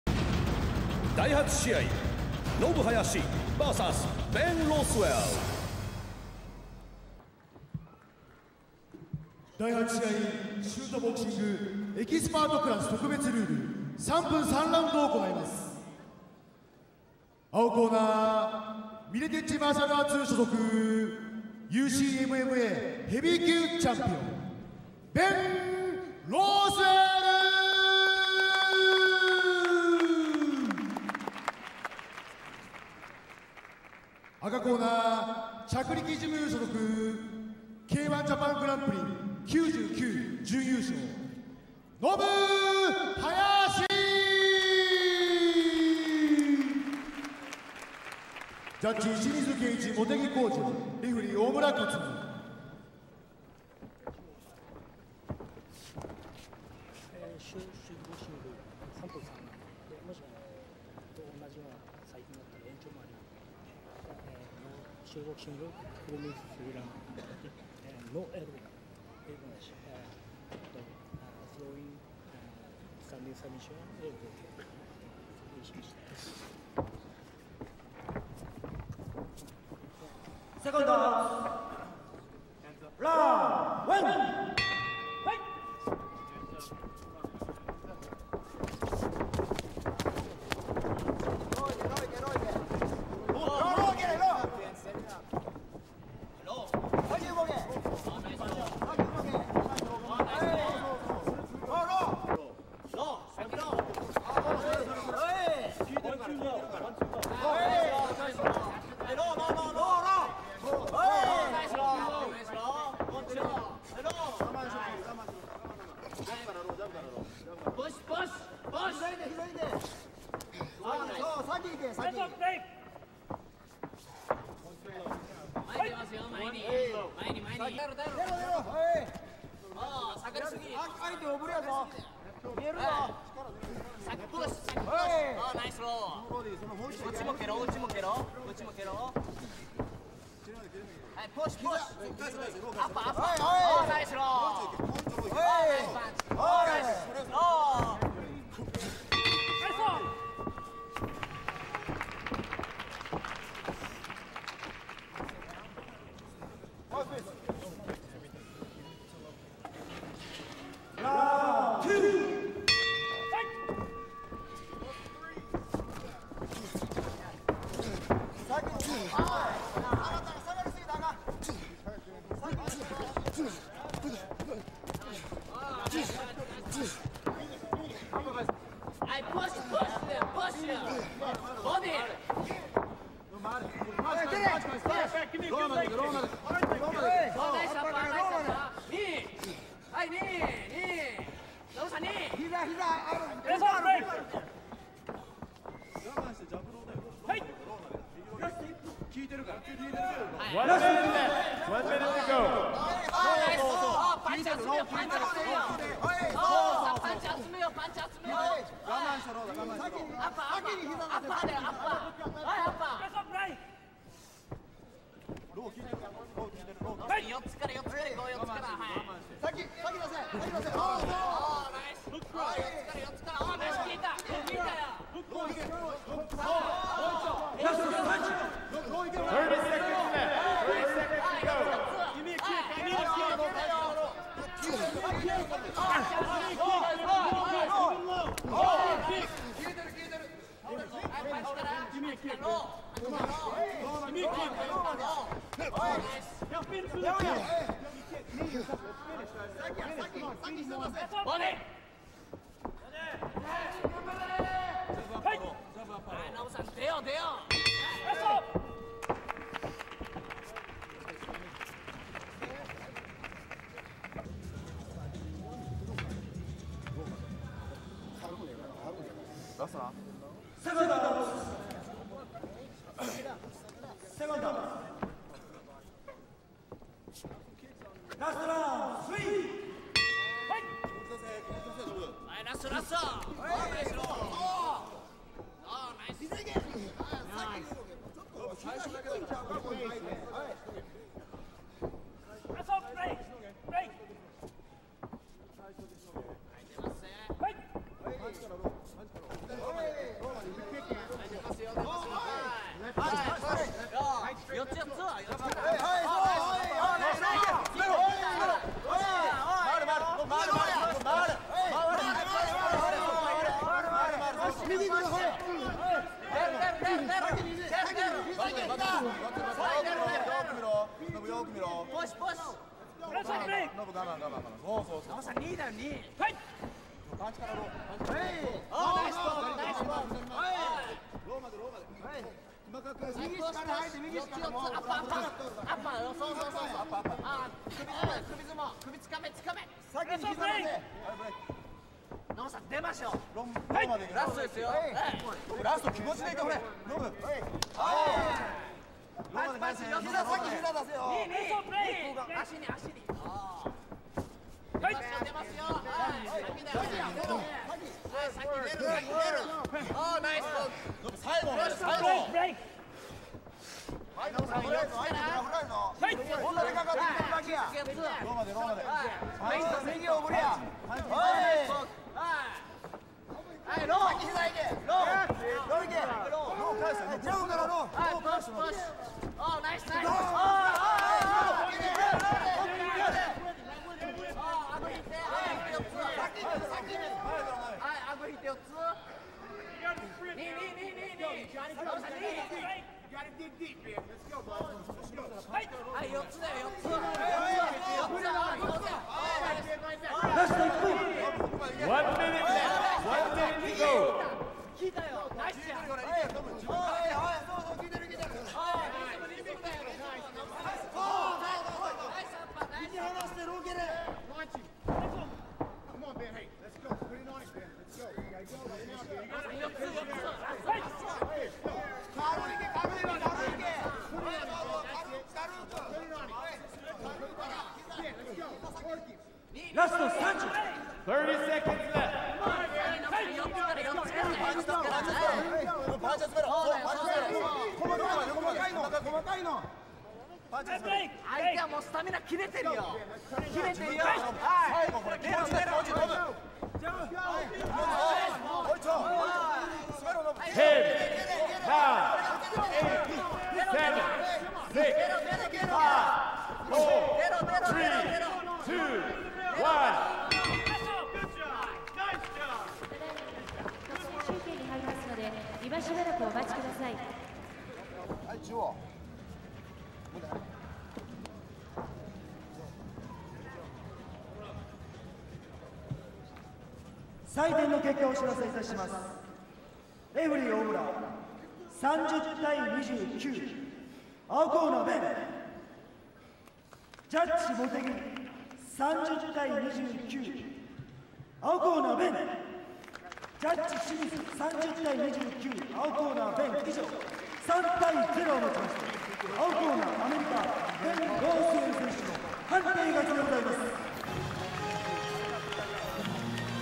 Ben Rothwell Vs. Nobu Hayashi (23/01/2005)
Ben Rothwell Vs. Nobu Hayashi
Shoot Boxing Ground Zero 2005
January 23, 2005 | Fukuoka, Japan

Heavyweight bout with modified Japanese rules.